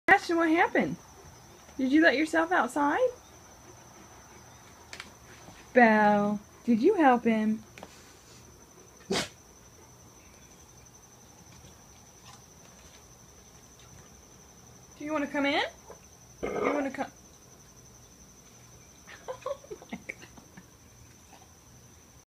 Great pyr burp
Loud burp from my 4 year old dog